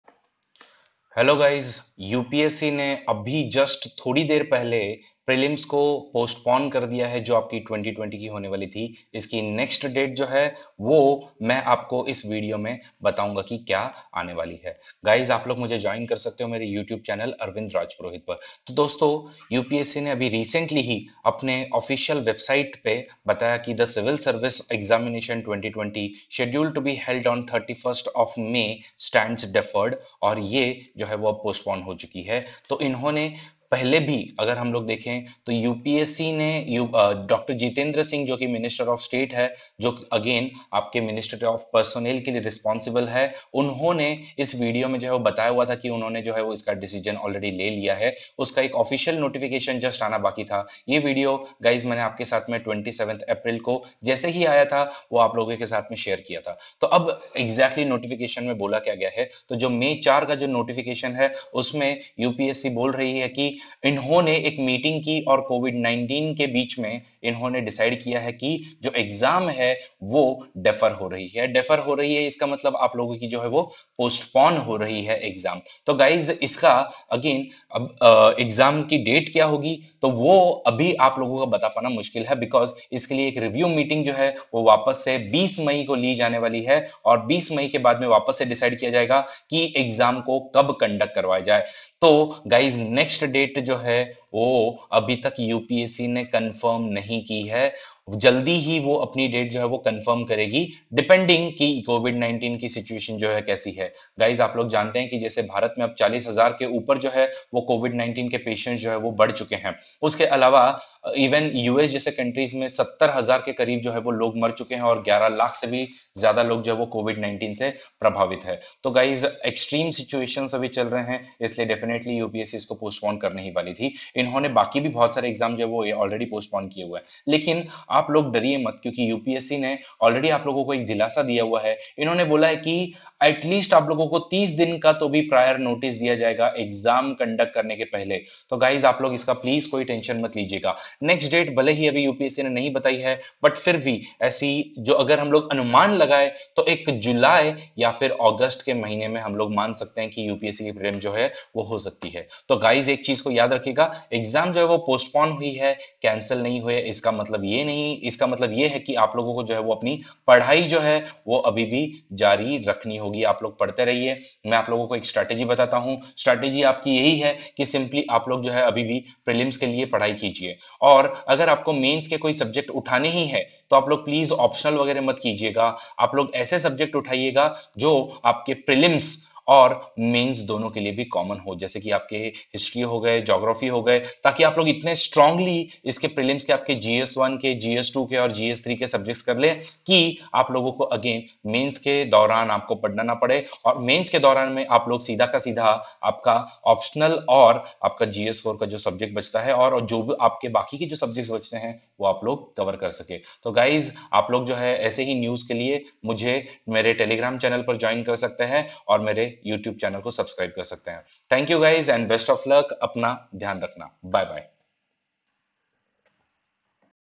UPSC CSE Prelim 2020 Postponed | New Date | Official Notification 4 May 2020 | By Arvind Rajpurohit
for latest date.